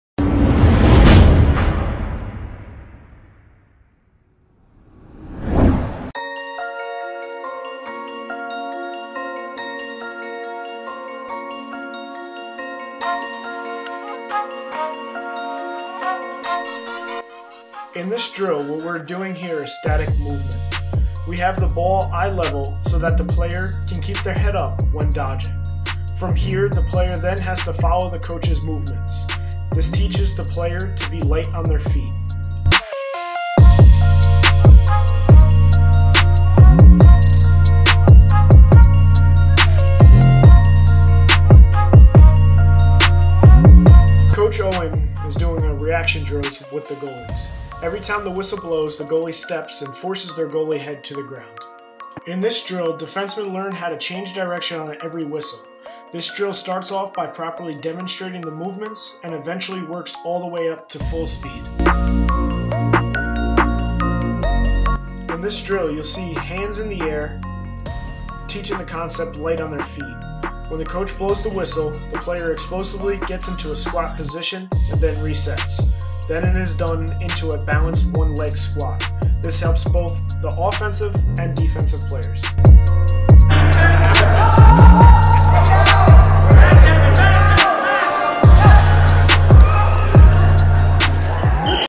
Winter Training
We break down our favorite drills with commentary.